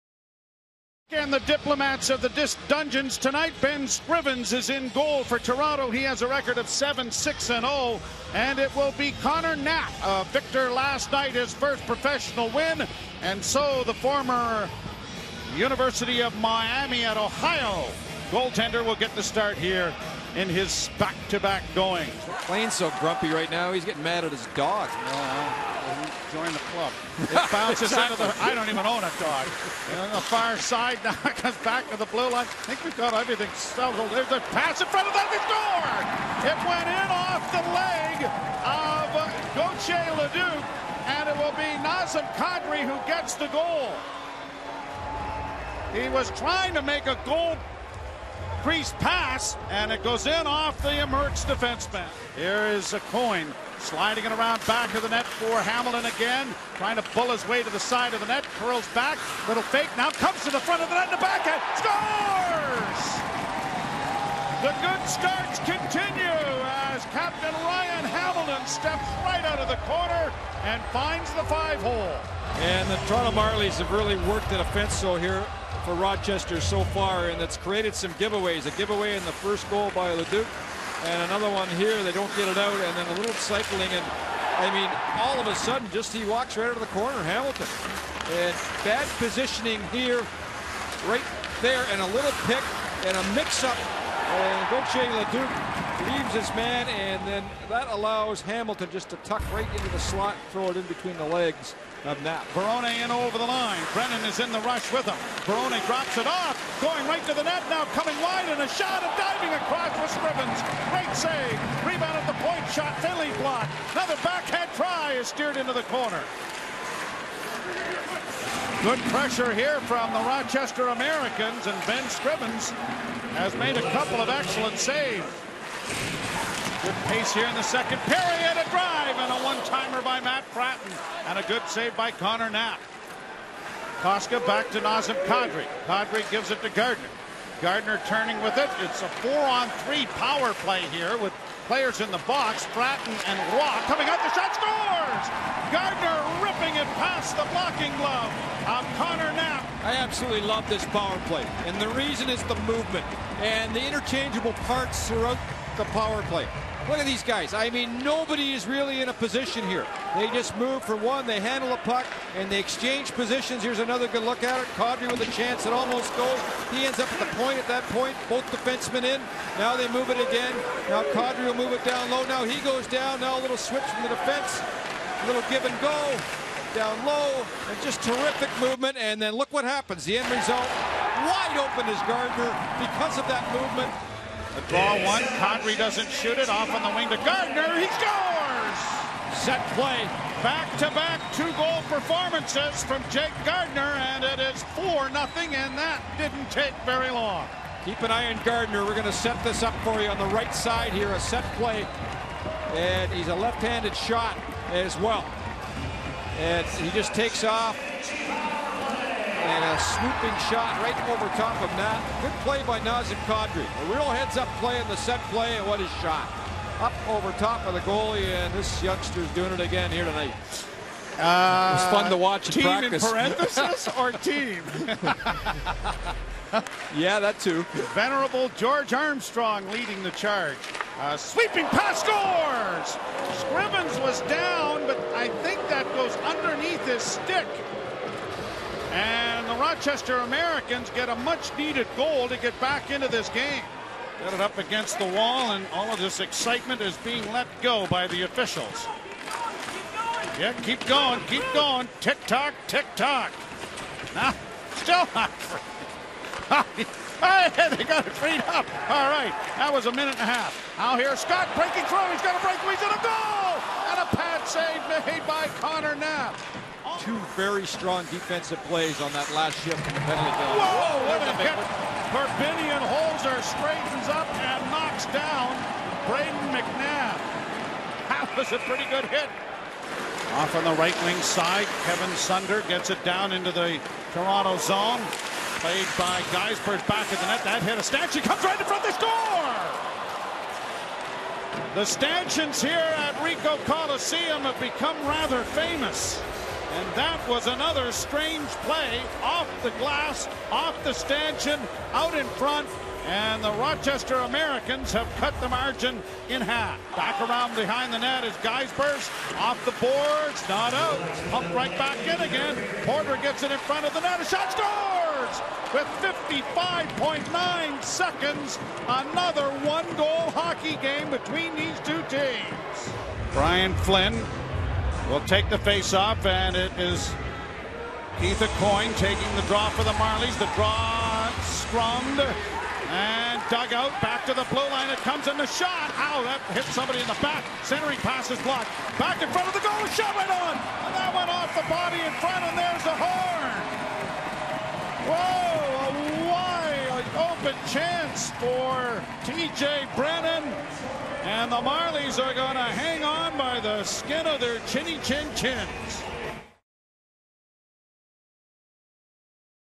Marlies in Six: Rochester at Toronto - 12/08/12
See all the highlights from the Marlies Saturday night win over Rochester, all in just six minutes.